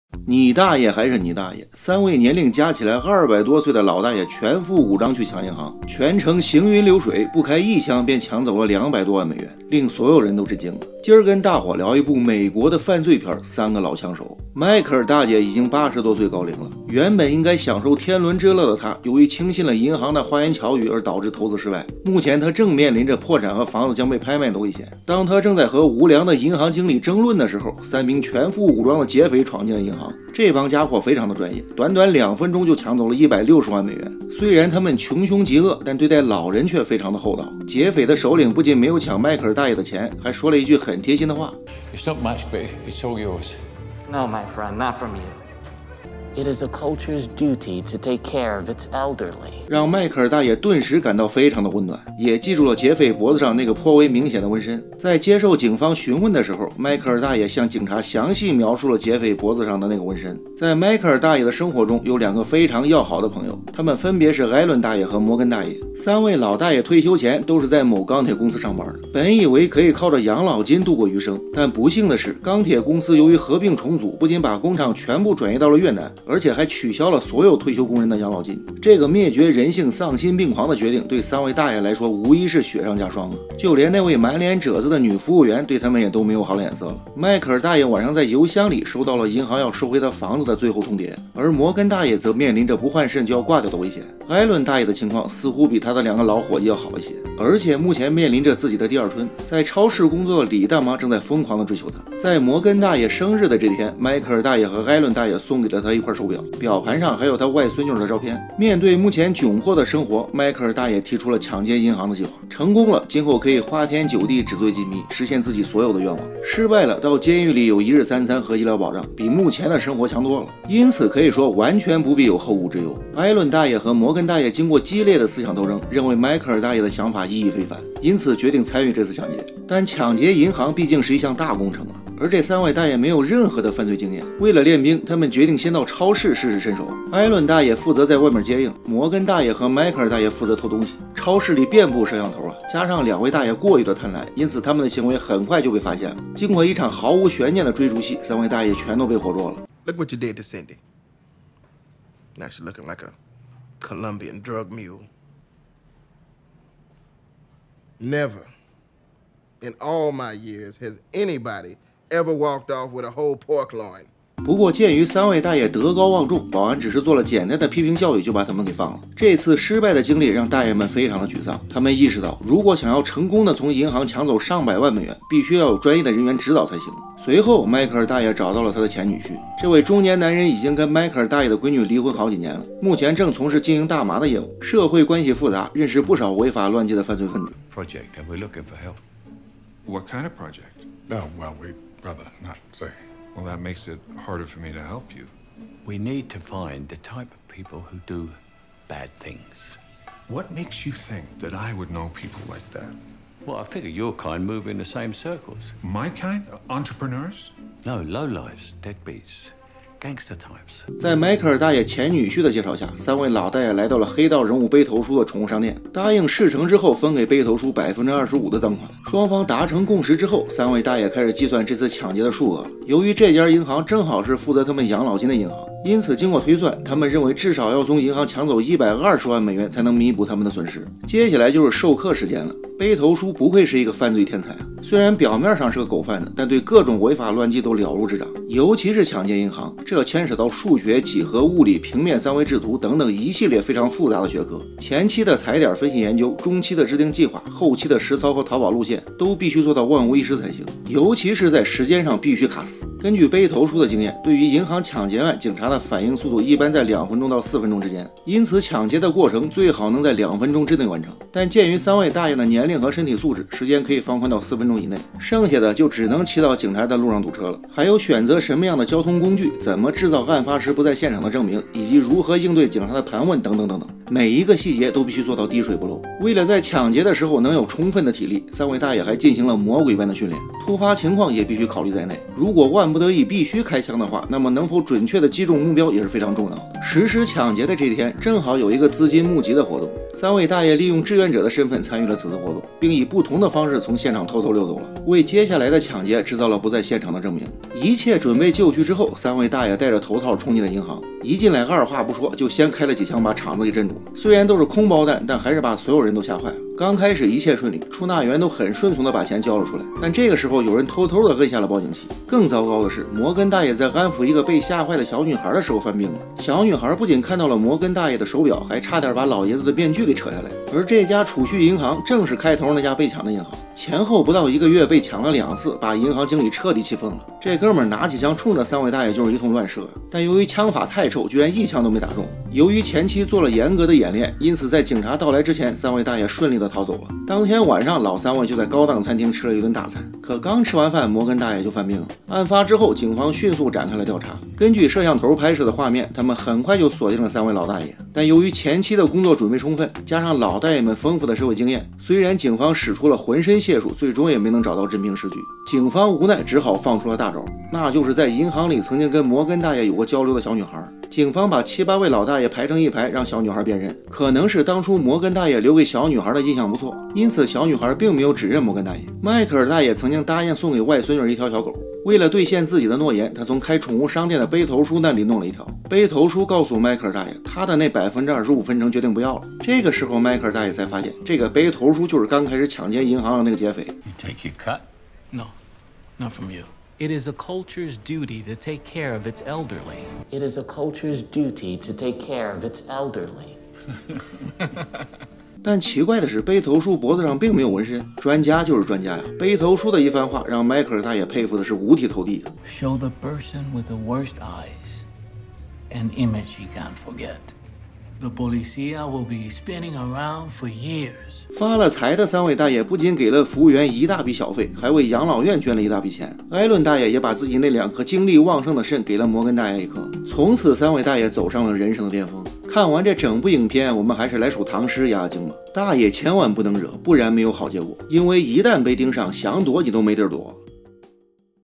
大爷千万不能惹，三位老大爷孤注一掷，还请了黑道专家当顾问
三位遵纪守法的好公民乔、亚尔、威利因为晚年生活凄凉，不得不铤而走险去打劫曼哈顿一家银行，成功抢劫了两百多万美金后，他们并没有因此而一帆风顺，反而引发了一系列故事